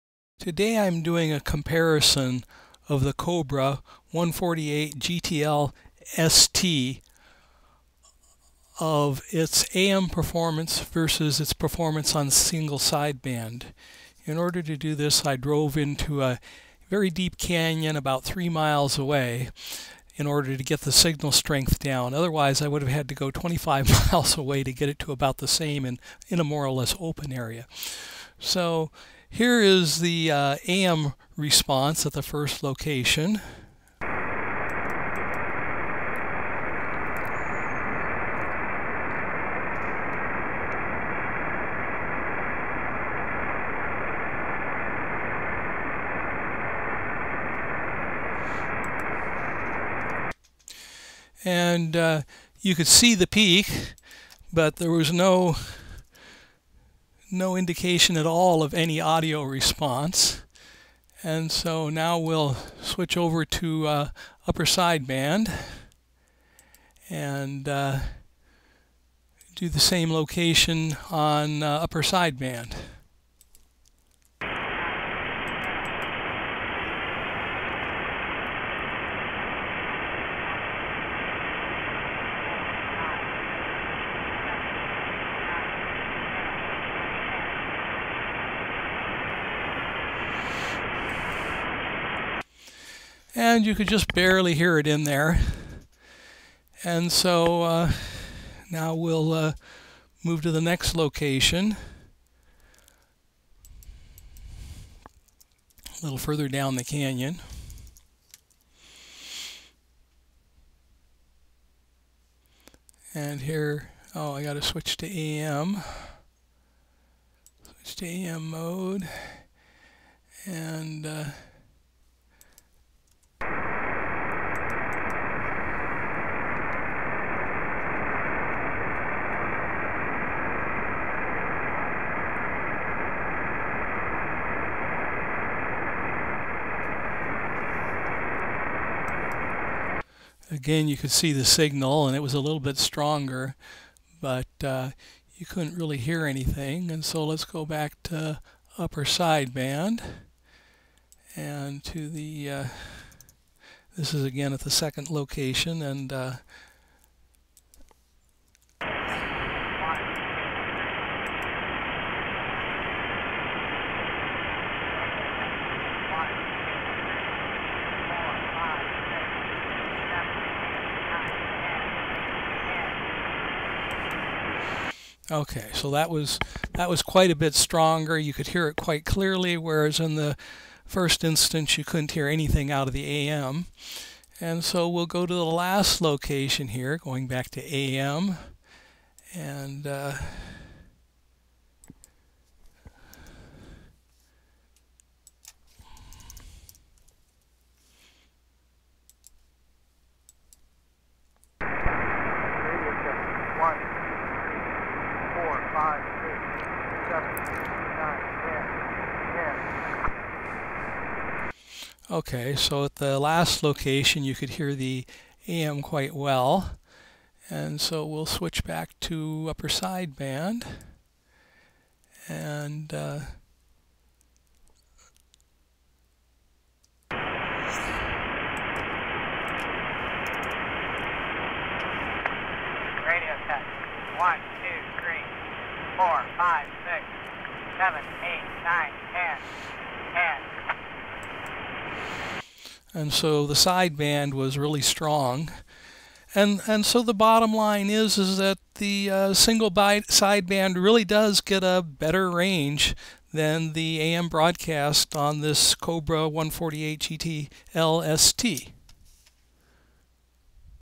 Cobra 148 GTL ST - AM vs USB at CB frequencies
I used SdrGlut to do a comparison of the Cobra 148 GTL ST using  AM vs using Upper SideBand. I tested it at three locations in a deep canyon about 3 miles away. Using USB I was able to hear the signal at all three locations, but I was able to hear the AM at only one location. The USB performance was far better than the AM performance. Now, if they would make a Software Defined Radio version of a SSB system - SSB would be easy to use and they might sell more radios.